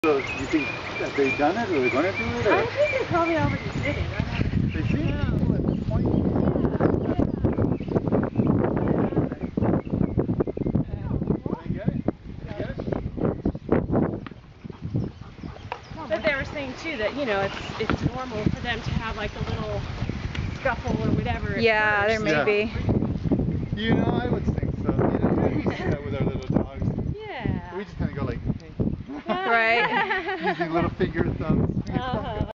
Molly and Manny Go Walking Together!
Molly & Manny enjoy a walk together on the SAME LEASH!